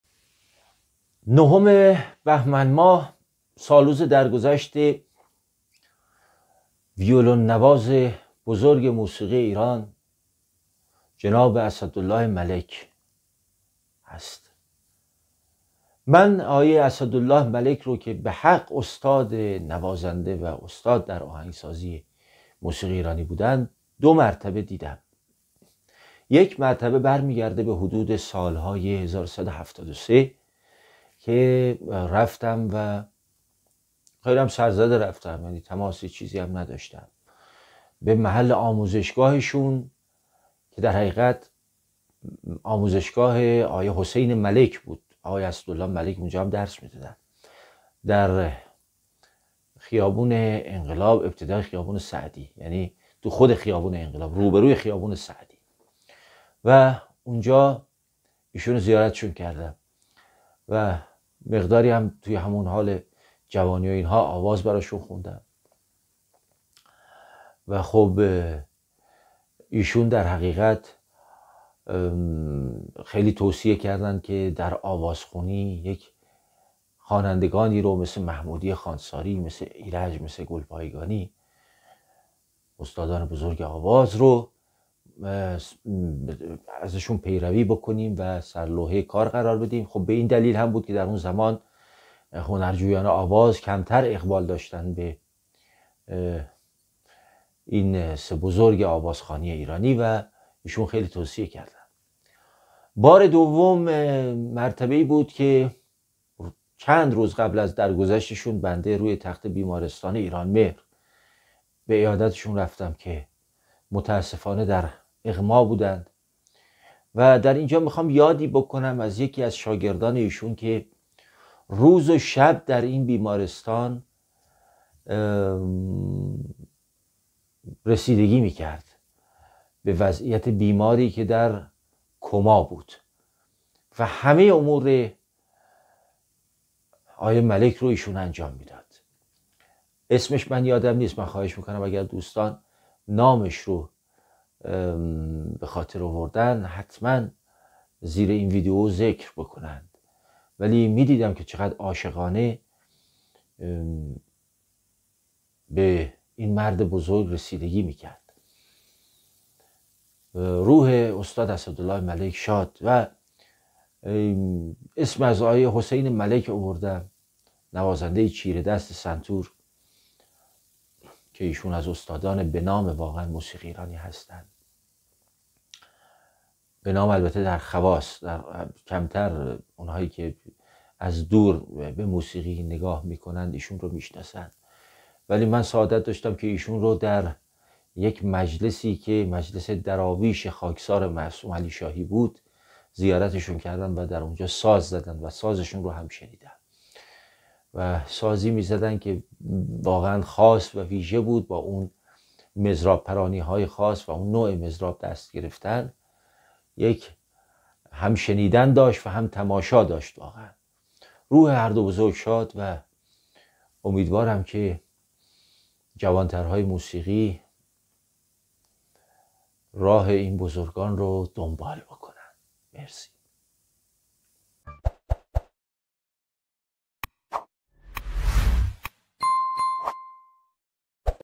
دیدار با اسدالله ملک در روزهای آخر-خاطرات موسیقی- روایت ۲۵ Meeting Assadollah Malek in the last days
اسداله ملک (زادهٔ ۱۴ مرداد ۱۳۲۰ – درگذشتهٔ ۹ بهمن ۱۳۸۰) از برجسته‌ترین نوازندگان ویولون، کمانچه و آهنگسازان و ملودی‌پردازان اهل ایران بود.

Assadollah Malek (born 14 August 1320 - died 9 February 1380) was one of the most prominent violinists, fiddlers, composers and melodists from Iran.

او نزد استادان بزرگی چون ابوالحسن صبا ، روح‌الله خالقی، شاپور نیاکان، حسین هنگ‌آفرین، حسین تهرانی و علی‌محمد خادم میثاق آموزش دیده بود و در ویولون‌نوازی، دارای سبک و سیاق ویژه و در بداهه‌پردازی، خوش‌ذوق و قریحه بود.

He had been trained by great masters such as Abolhassan Saba, Ruhollah Khaleghi, Shapoor Niakan, Hossein Hongafarin, Hossein Tehrani and Ali Mohammad Khadem Misagh.

در این ویدیو حسین علیشاپور به مناسبت نهم بهمن ماه سالگرد درگذشت این استاد موسیقی ایرانی، یادی از او و حسین ملک برادر بزرگتر ایشان (نوازنده برجسته‌ی سنتور) کرده است.

In this video, Hossein Alishapour, on the occasion of the 9th of Bahman, the anniversary of the death of this Iranian music master, remembers him and his older brother Hossein Malek (a prominent dulcimer).

ضبط و ادیت ویدیو: سپیده_نیک‌رو
Video recording and editing

پس از شنیدن این ویدیو لطفاً کانال ما را سابسکرایت کنید.

دانلود قانونی آثار حسین_علیشاپور
Legal download of Hossein_Alishapour Works